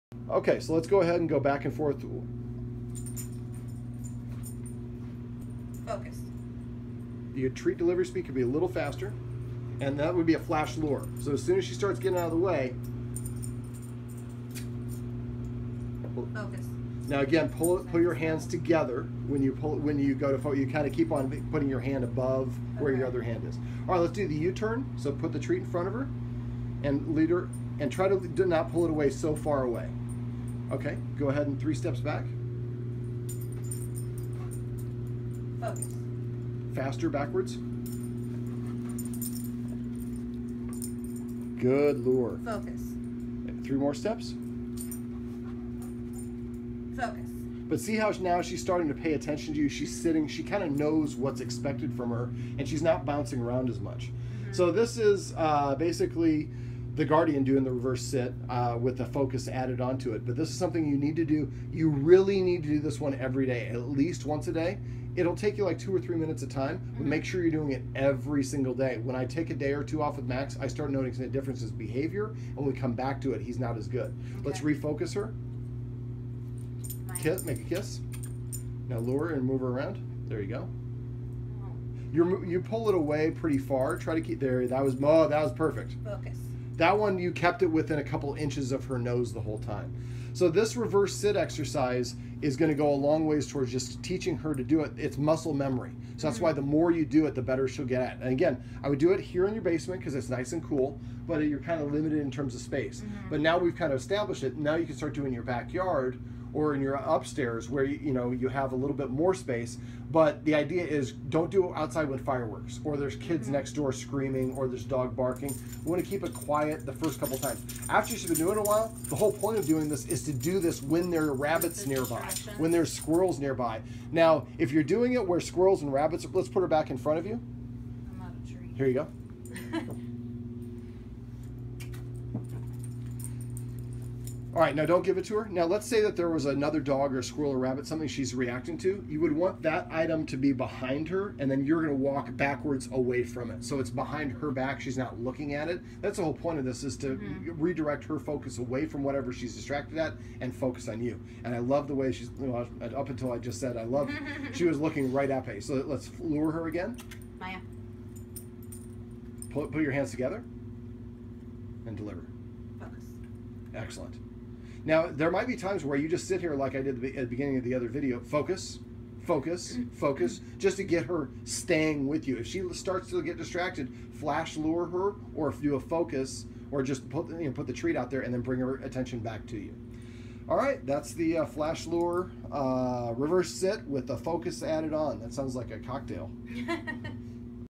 Maya's Guardian Practices the Reverse Sit
Maya is a one-year-old German Shorthair Pointer Lab mix who I worked with about a year ago. Her guardian scheduled a follow up dog behavior session due to Maya’s progressive decrease of responsiveness to commands and corrections.

This is one of the videos we shot during her dog obedience training session.